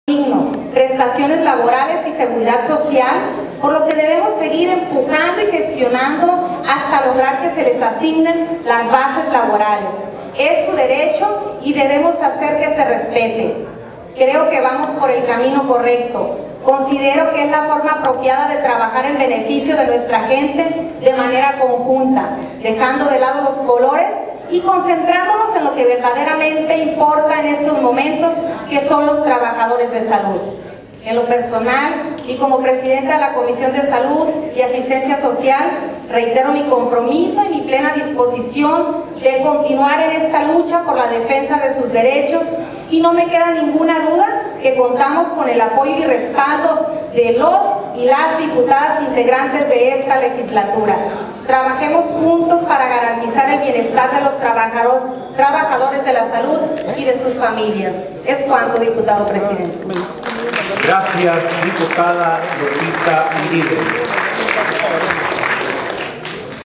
Diputada Lupe Iriben apoya a trabajadores de salubridad SSA en sus derechos laborales
CULIACAN, SINALOA, CONGRESO LOCAL